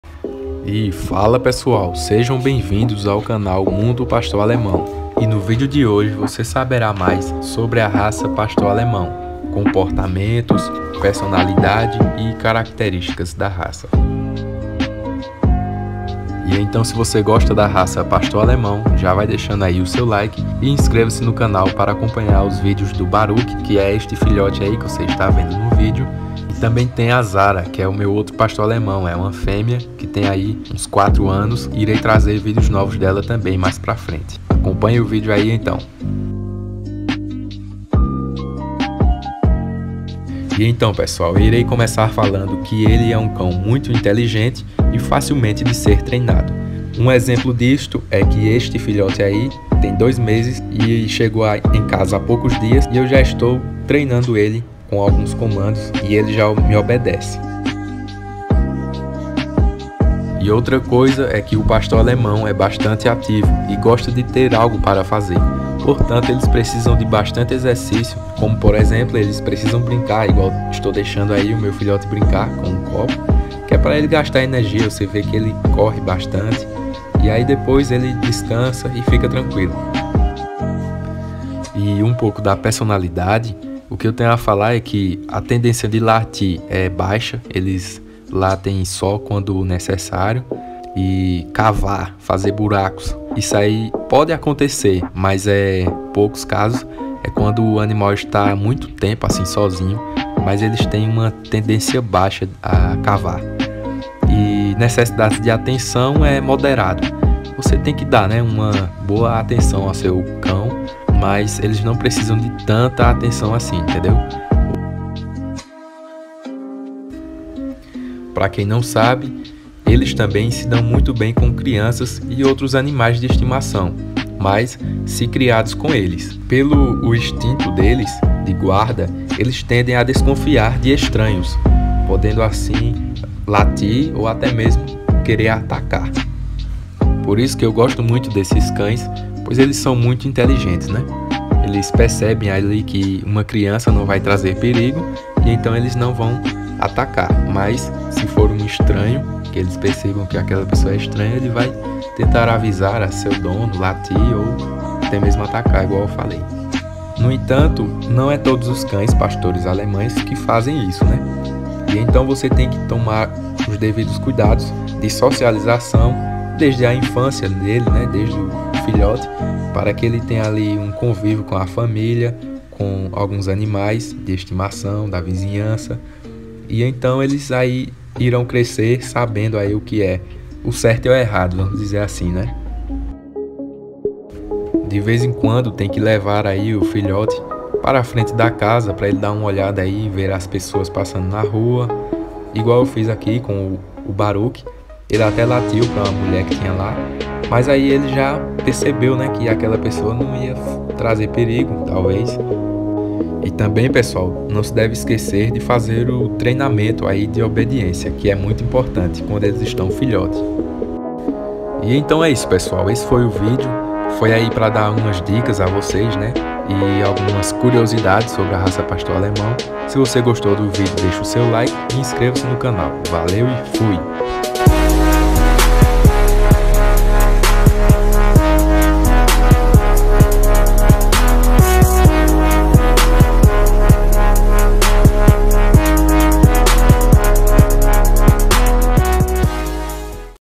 ASSISTA SE VOCÊ TEM OU QUER TER UM PASTOR ALEMÃO
Pastor Alemão e suas características. Saiba tudo sobre a raça pastor alemão! Características; Comportamento e Temperamento.

O cachorro Pastor alemão certamente é um dos mais elegantes e ativos cães. Apesar de ser capaz de amedrontar quem não o conhece, é muito dócil com seus tutores e, se bem treinado, é brincalhão quando permitido.

Muito obediente, está sempre alerta para proteger a casa e a família. Se acostumado desde cedo, pode ser uma excelente companhia a crianças e se dará muito bem com outros animais.

Certamente resignação e lealdade são suas principais características, mas são capazes de alegrar muito a casa com brincadeiras enérgicas e animadas!

Condição física: Antes de saber em detalhe tudo sobre Pastor alemão, é importante ficar por dentro das necessidades que esse cachorro tem quanto à rotina. Por serem muito ativos, de caça e pastoreio, os Pastores alemães precisam se exercitar diariamente, correr e fazer atividades que exijam sua inteligência e perspicácia para evitar o tédio. Caminhadas e brincadeiras com os tutores são ótimas práticas!